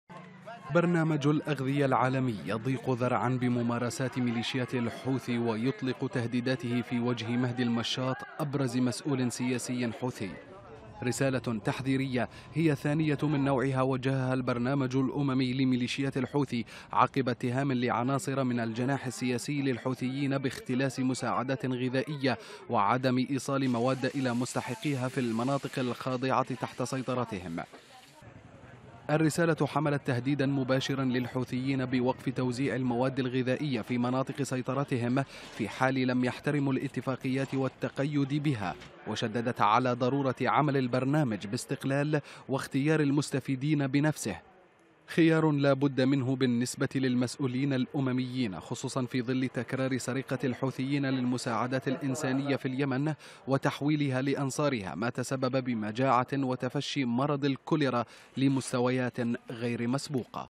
منظمة أممية كبيرة تتهم الحوثيين بالسرقة
هدد برنامجُ الغذاء العالمي  بوقف تقديم المساعدات الغذائية بعد عملياتِ سرقةٍ واختلاس قامت بها عناصرُ من الجناح السياسي لميلشيات الحوثي، ويُعد التهديدُ الأممي الثاني من نوعه في غضون أشهر.